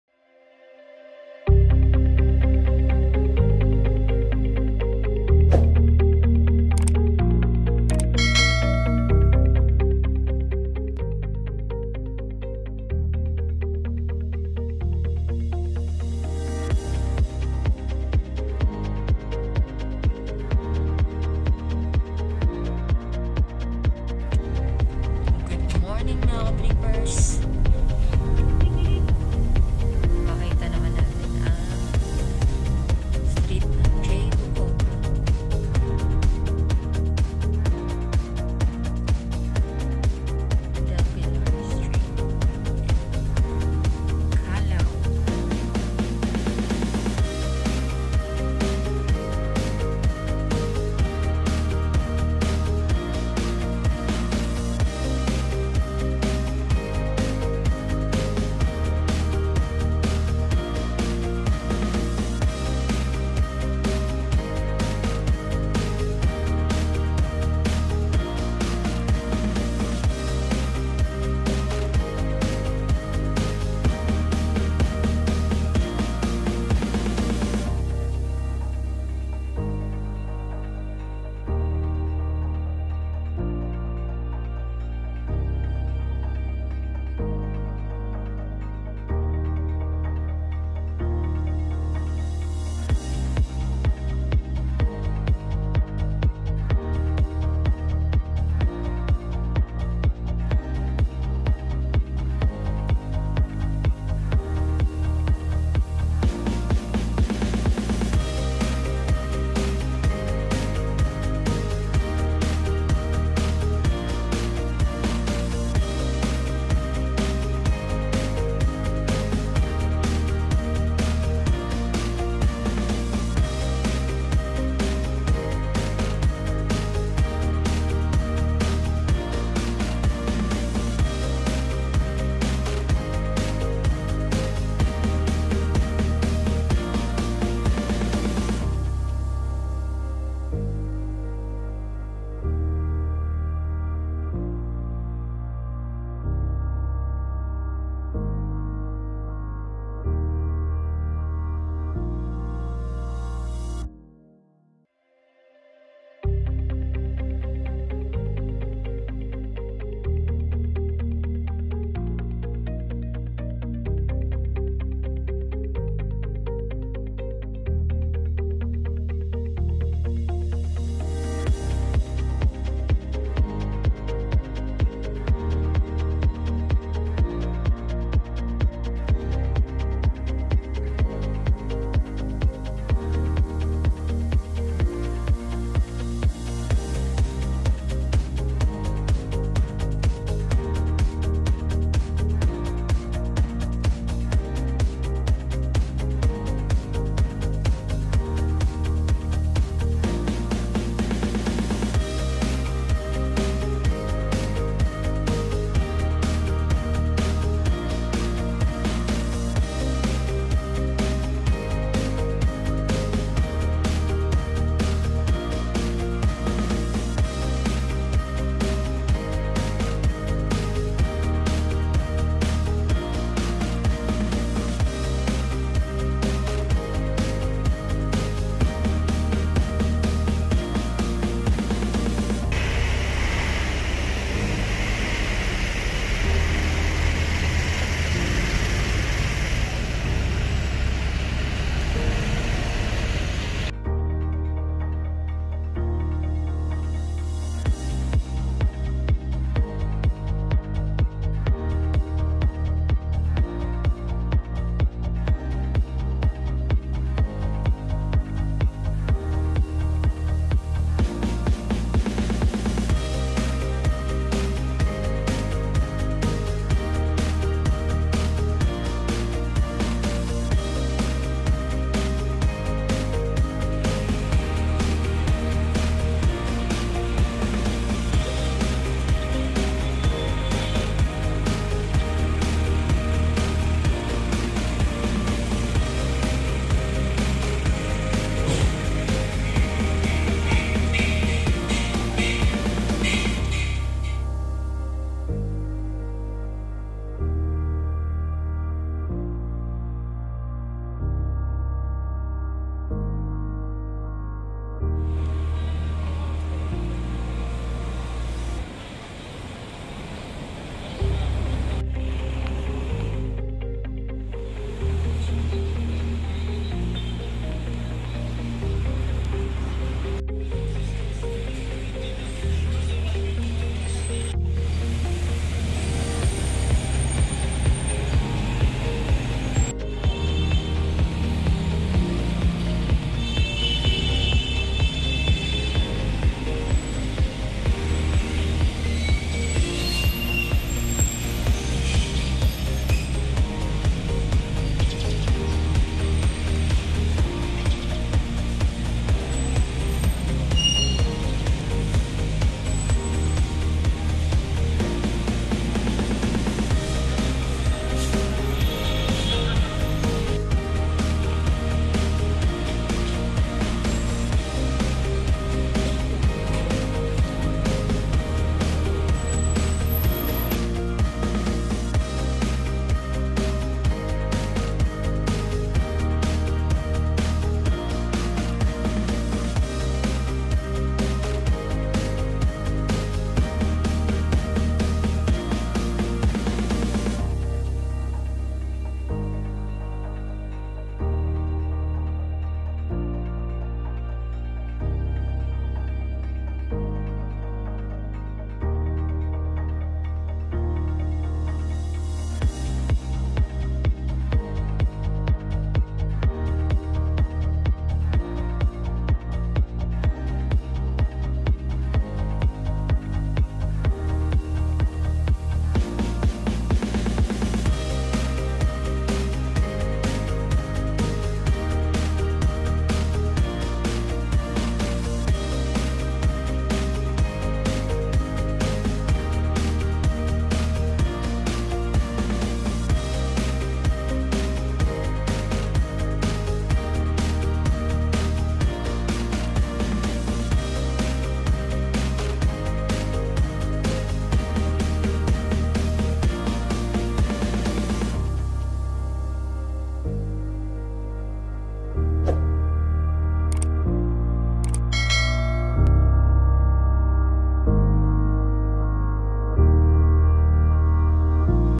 JORGE BOCOBO ST - J BOCOBO ST - ERMITA - MANILA (Roxas Blvd-Kalaw-UN Ave-Padre Faura) - 08.Feb.2022
Drive-walk at J Bocobo Street Ermita Manila 👍

Live.Love.Laugh 👫💚🥳